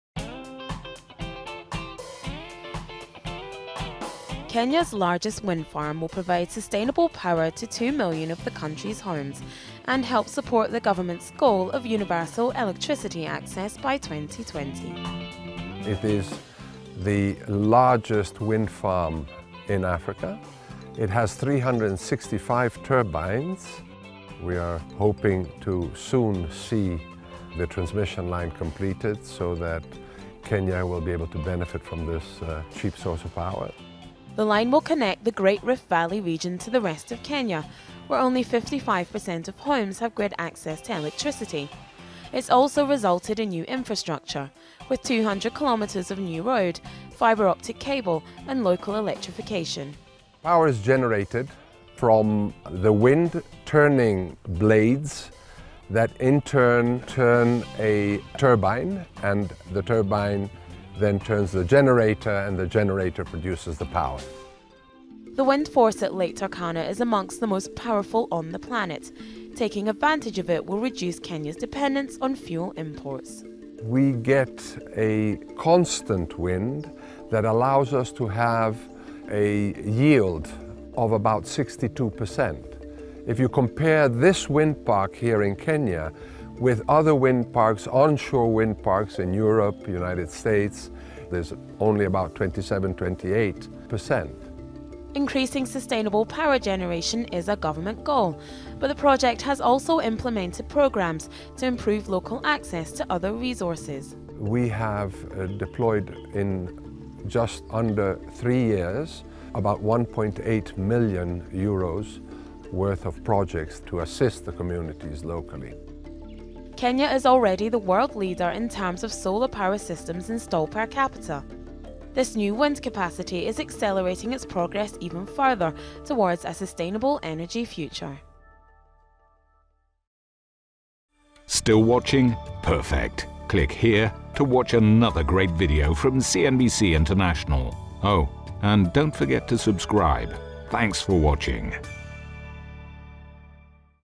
A wind farm of epic proportions is taking shape in Africa | Sustainable Energy
In Africa, efforts to generate wind energy on a grand scale are underway.

CNBC_international